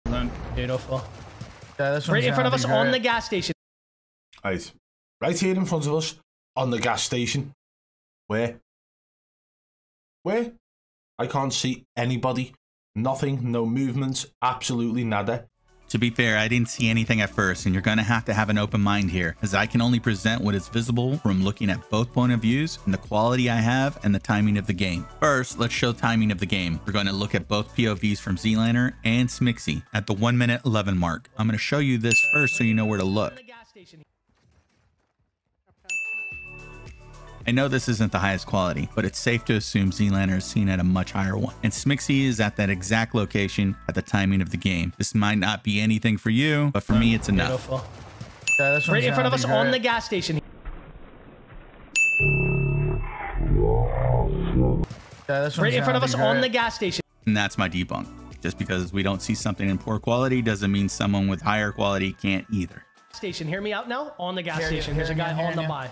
ZLANER CHEATING IN WSOW QUALIFIERS!? (Debunked)
To be fair I didn't see anything at first and your going to have to have an open mind here as I can only present what is visible from looking at POV with the quality I have and the timing of the game.